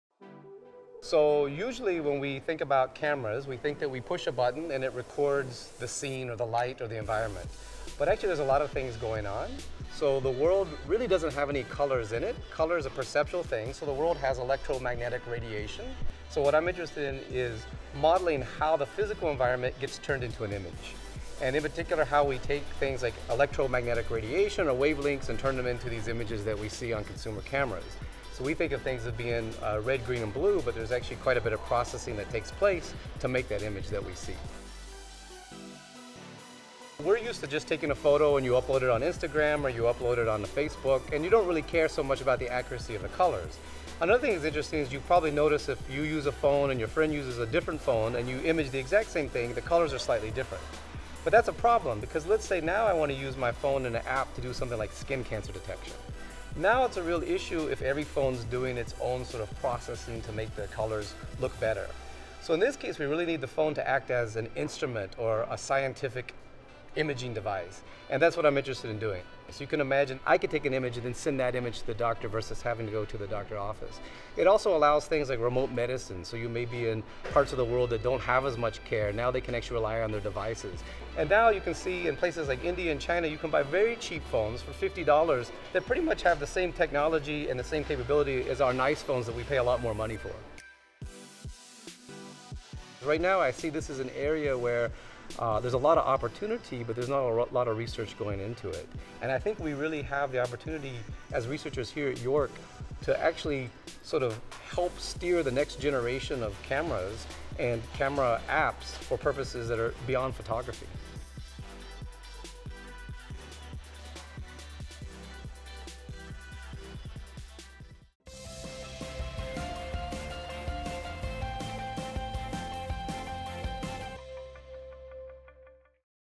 Do You See What I See?
Every phone camera captures colour a little bit differently, which can be trouble if you're using one to get a diagnosis.